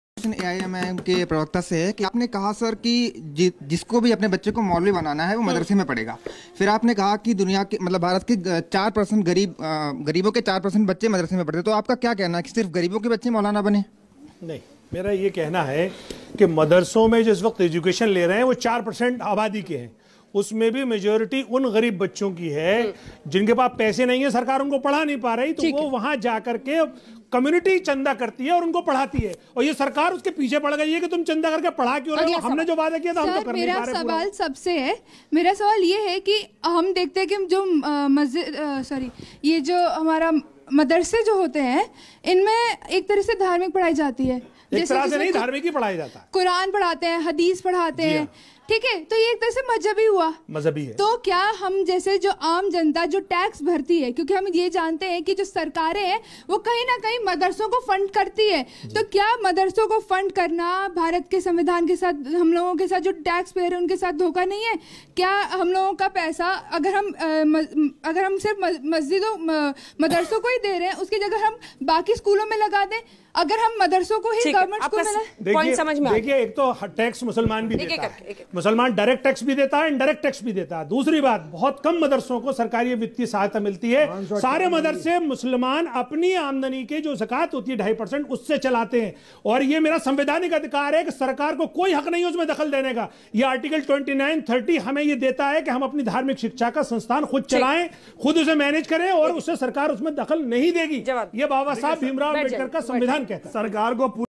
मुसलमान भी सरकार को टैक्स देते हैं फिर भी सरकार मदरसों को बहुत कम वित्तीय सहायता देती है:
मुसलमान भी सरकार को डायरेक्ट और इनडायरेक्ट टैक्स पे करते हैं फिर भी सरकार मदरसों को बहुत कम वित्तीय सहायता देती है, ज़्यादातर मदरसे क़ौम अपने चंदे से चलाती है।